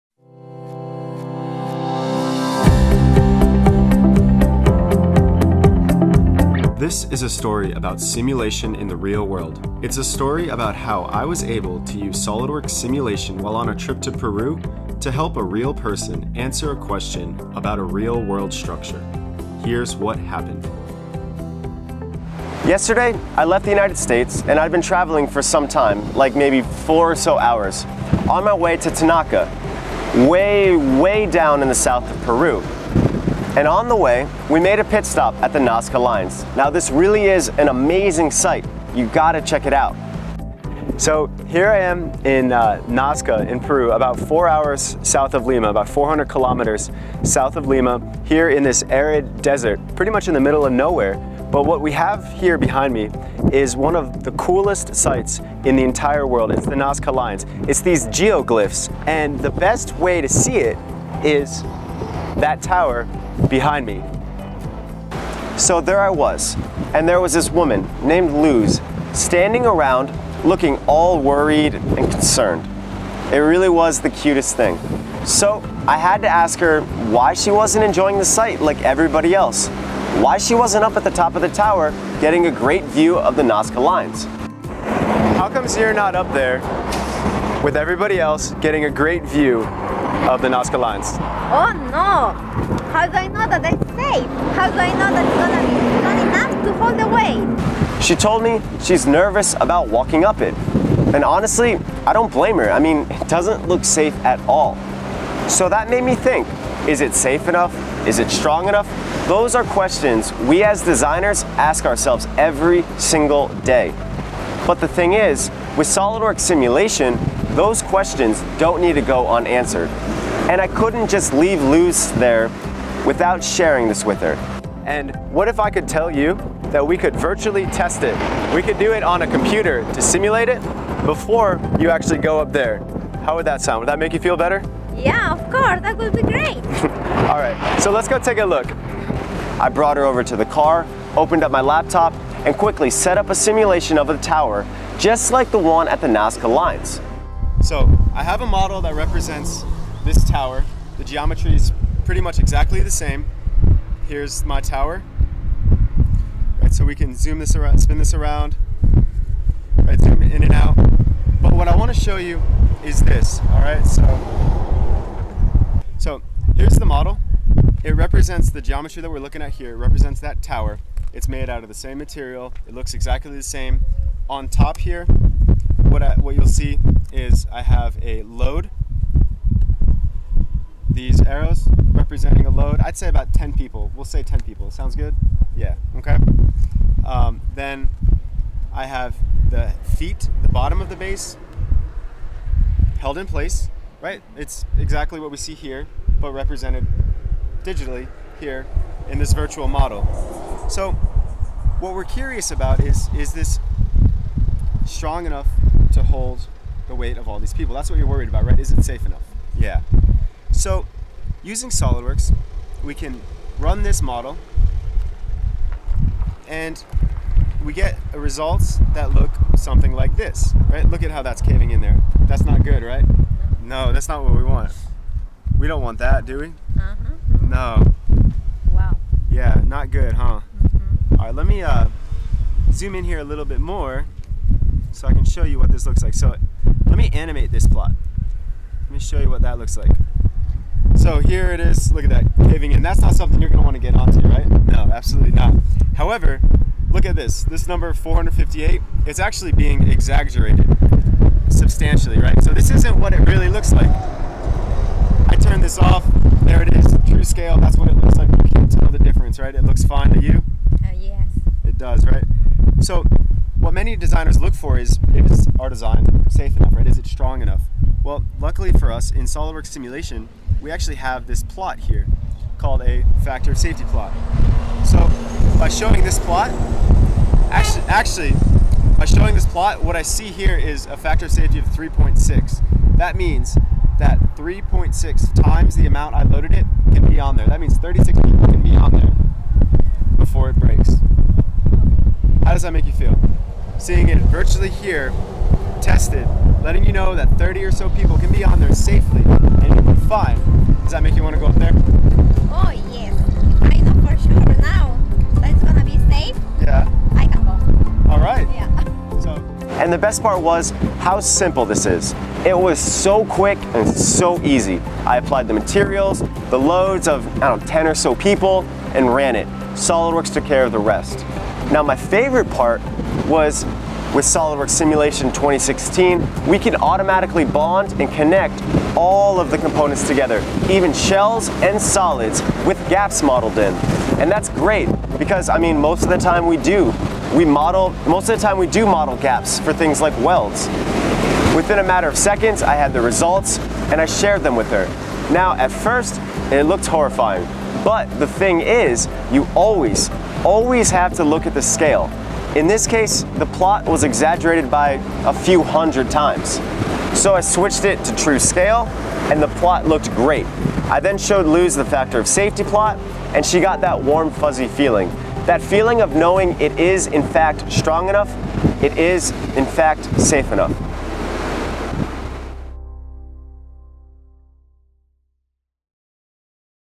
Video Tech Tip: SOLIDWORKS Simulation in the Real World: Peru
Is it safe enough? Is it strong enough? We always have engineering on the mind at TriMech. Application Engineer Stephen Petrock found himself answering these questions using SOLIDWORKS Simulation while on a recent trip to Peru! Watch him do a quick safety factor check of the observation tower at the Nazca Lines.